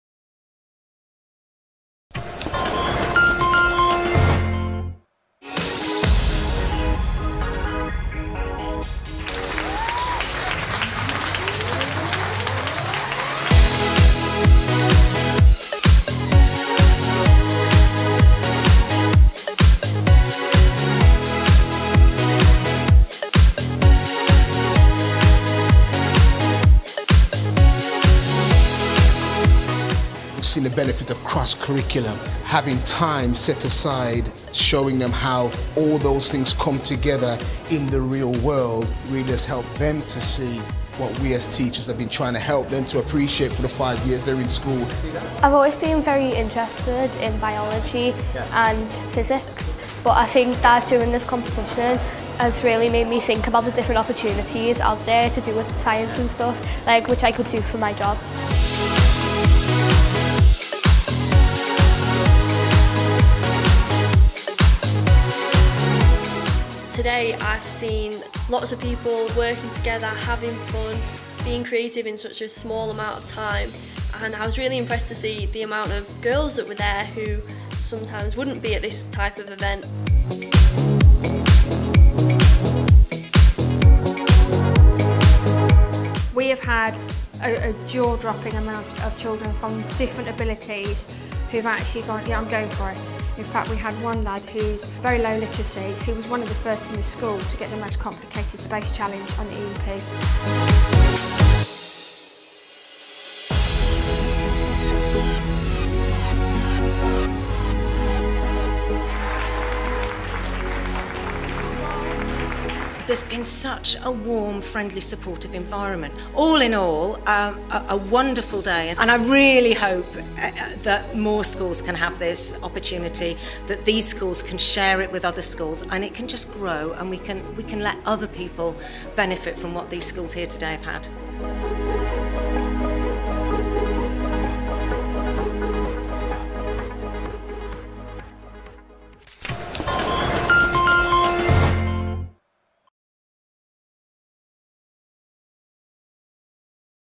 The Tomorrow's Engineers EEP Robotics Challenge
Schools across the country are invited to take on a robotics challenge that will see student teams involved in ‘space missions’.

Teamwork, robots, design, discovery, fun and loads of LEGO are all part of the mix, as are real-world challenges, teacher support and some great prizes.

Are you ready to take your students on a journey of exploration?